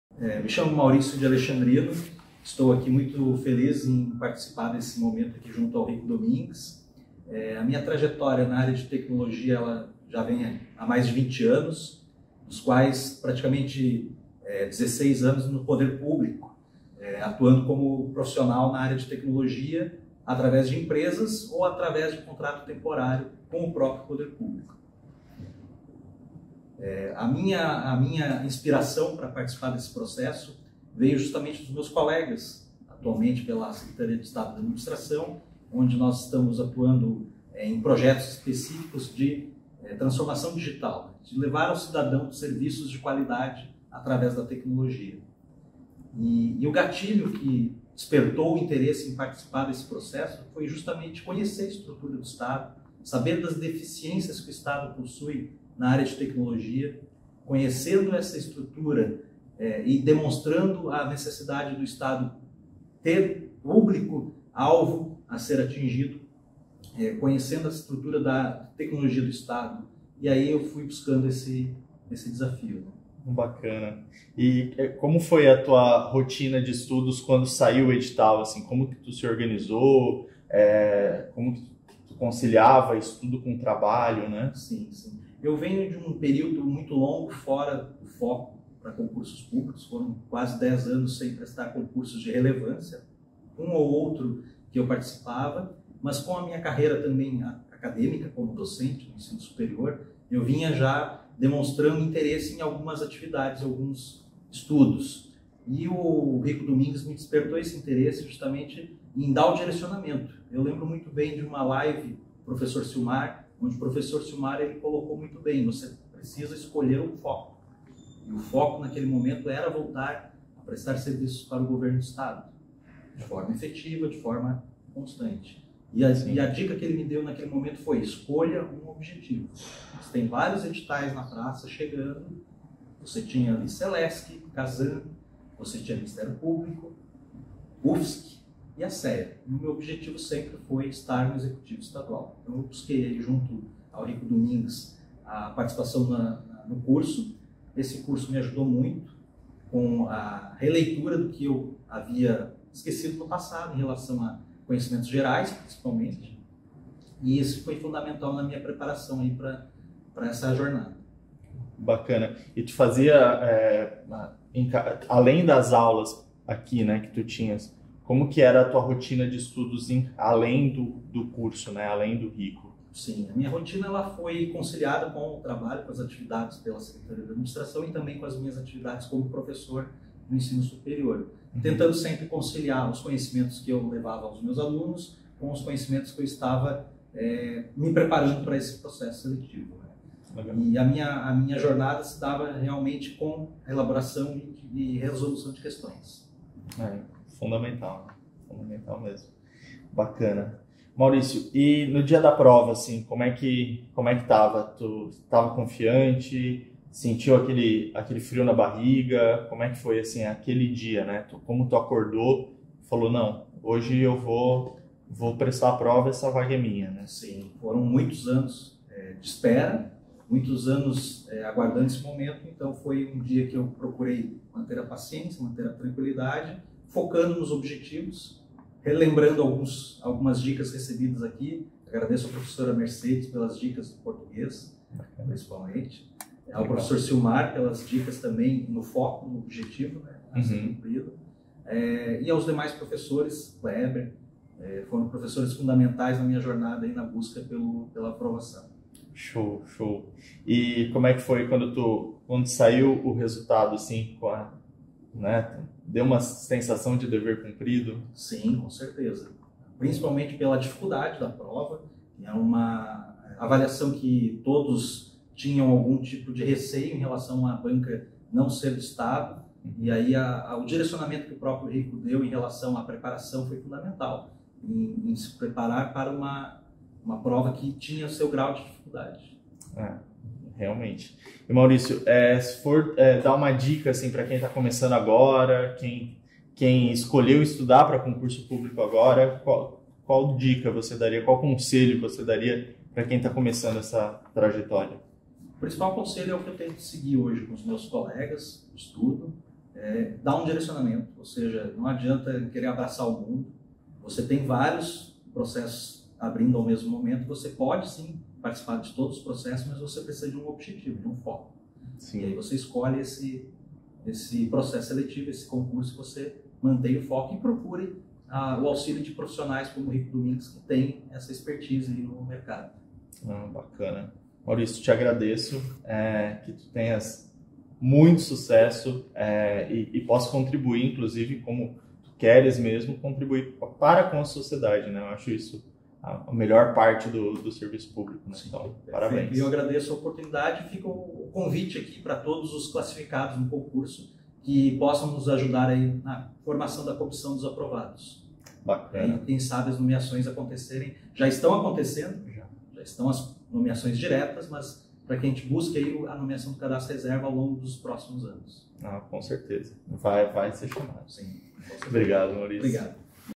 Inspire-se com o aprovado no concurso SEA/SC - Mauricio
Depoimento do nosso ex-aluno Mauricio.